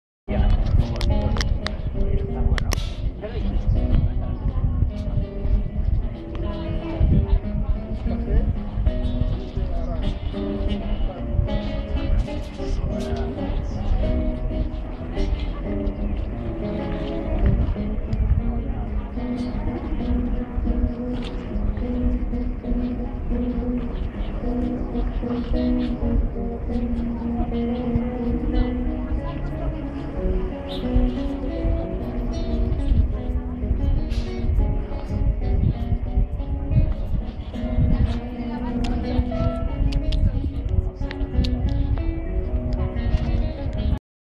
Parque del Retiro Madrid
Esto es lo q andaba buscando...
Mood: Relax
Lugar: Parque del Retiro, Madrid - España
Fecha: 16 de Septiembre de 2011